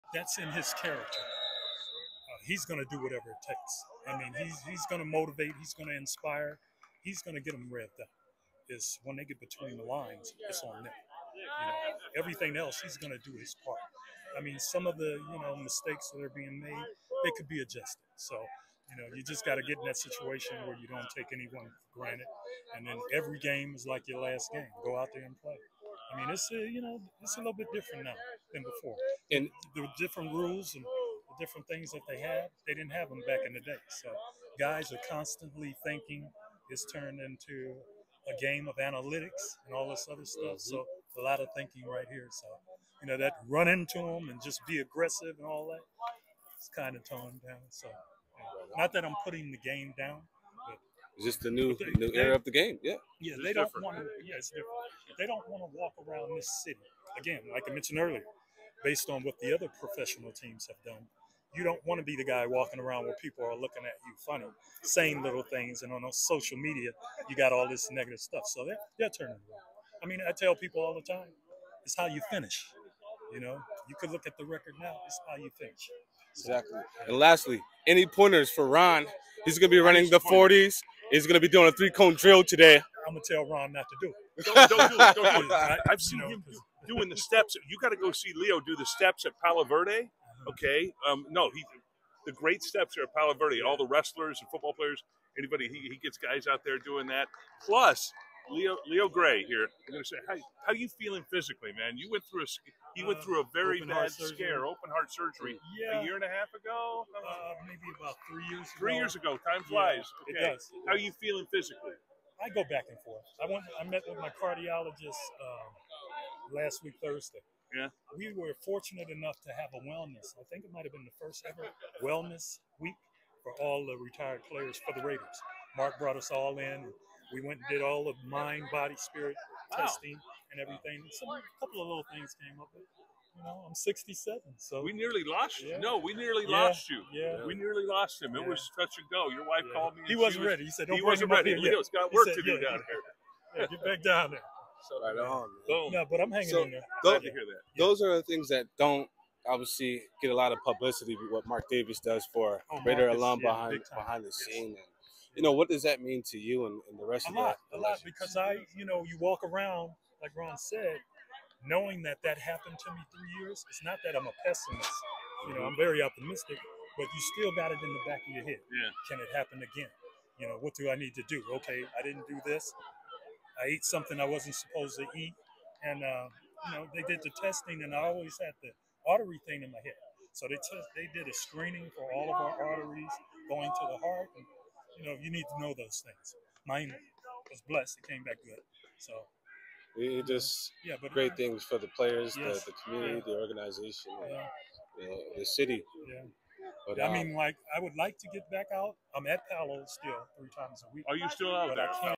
MATT SLUKA LEAVES UNLV FOOTBALL; RAIDERS LEO GRAY TALKS UNLV & RAIDERS
UNLV starting quarterback Matthew Sluka is stepping away from the Rebels program and will redshirt with intentions of entering the transfer portal, he announced late Tuesday on social media. Sluka attributed the leave to "certain representations" that the school made to him when he transferred from Holy Cross this offseason.

"I have decided to utilize my redshirt year and will not be playing in any additional games this season," Sluka said. "I committed to UNLV based on certain representations that were made to me, which were not upheld after I enrolled. Despite discussions, it became clear that these commitments would not be fulfilled in the future. I wish my teammates the best of luck this season and hope for the continued success of the program."